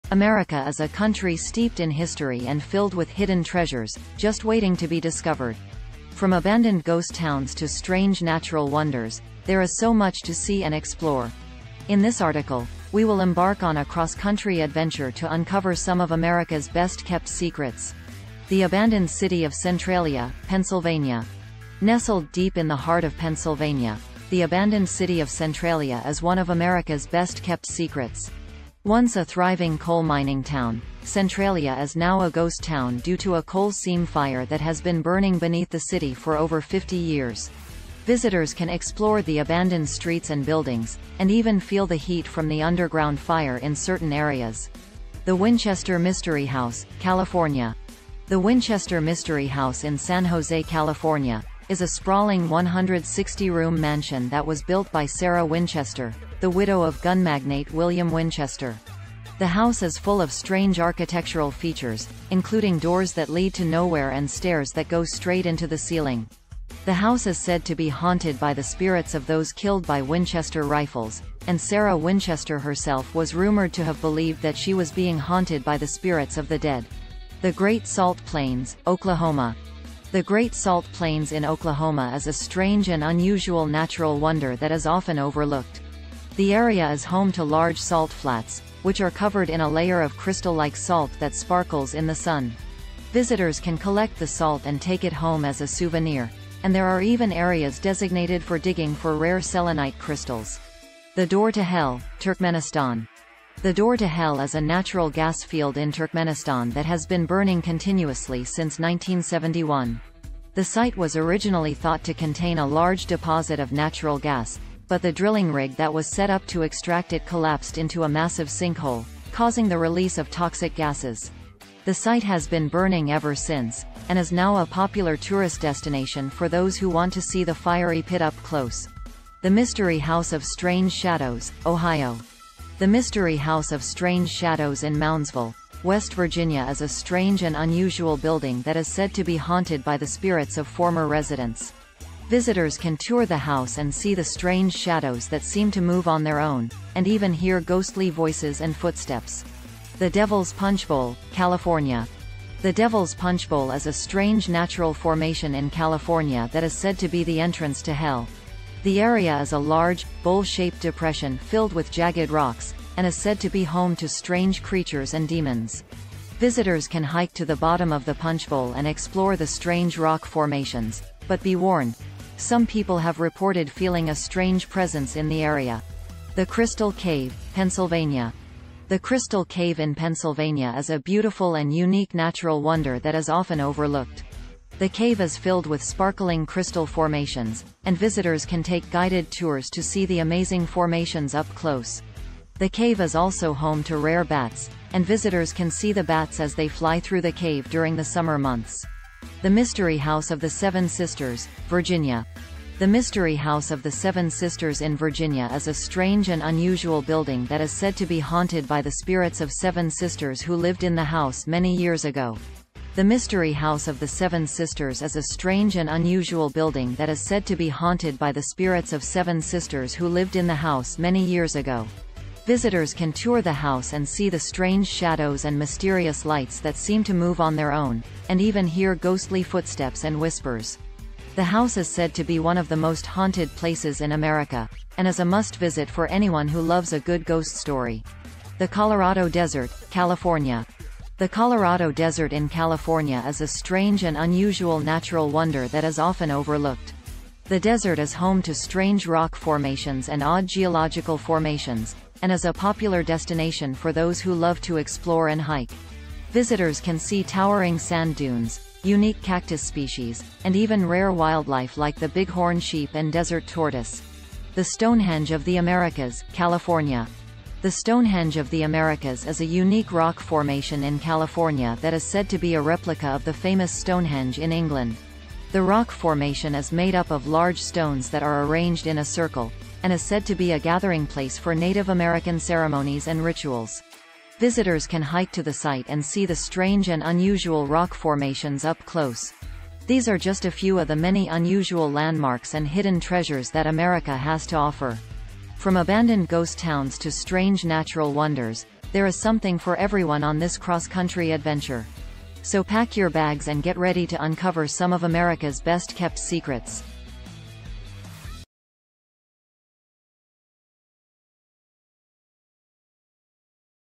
Uncover America's Best Kept Secrets: A Cross-Country Adventure".
Embark on a journey to uncover some of America's best kept secrets and unusual landmarks. From the abandoned city of Centralia, Pennsylvania to the mysterious Winchester Mystery House in California, this adventure takes you on a cross-country tour of strange and fascinating destinations. Visit the Great Salt Plains in Oklahoma, the Door to Hell in Turkmenistan, and the Mystery House of Strange Shadows in West Virginia. Explore the Devil's Punchbowl, the Crystal Cave in Pennsylvania, and the Mystery House of the Seven Sisters in Virginia. Discover the Colorado Desert and the Stonehenge of the Americas in California. Book your trip today and discover the hidden treasures of America!